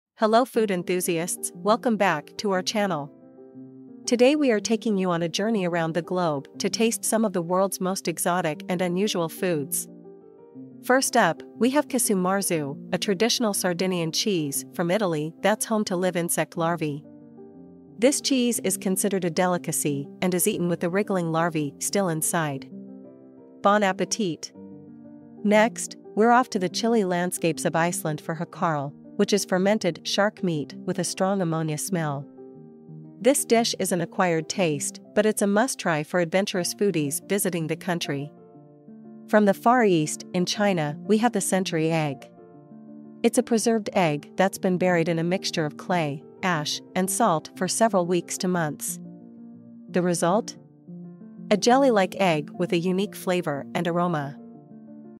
The World's Most Exotic And Unusual Foods
In this hilarious and educational video, we take a look at some of the most fascinating and unusual foods in the world! From dragon's blood coffee to yak milk tea, we take a journey to some of the world's most exotic and unusual destinations.

Casu Marzu - This cheese, which comes from Sardinia, Italy, is unique for being home to live insect larvae, specifically cheese fly larvae. The larvae help break down the fats in the cheese, making it very soft. The cheese has a strong flavor, and is traditionally spread on flatbread.

Hákarl - This Icelandic dish is made from shark meat that's been fermented and hung to dry for several months. It has a strong smell of ammonia and a distinctive fishy taste. Hákarl is often served in cubes on toothpicks.

Century Egg - Also known as a preserved egg or thousand-year egg, this Chinese delicacy is made by preserving duck, chicken, or quail eggs in a mixture of clay, ash, salt, quicklime, and rice hulls for several weeks to months. The yolk of the egg turns dark green or even black and has a creamy consistency, while the white becomes a dark amber color and has a jelly-like texture.